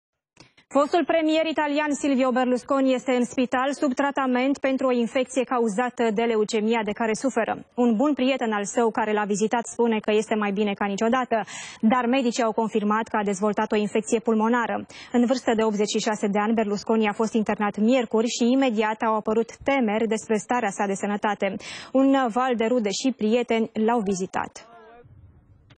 Medicul curant al lui Berlusconi: Este o situaţie dificilă, dar răspunde bine la tratament
Doctorul Alberto Zangrillo, medicul curant al lui Silvio Berlusconi, a recunoscut sâmbătă că fostul premier italian suferă de "o patologie gravă" şi se află într-o situaţie "dificilă", dar a subliniat că "răspunde bine la tratament", transmite EFE.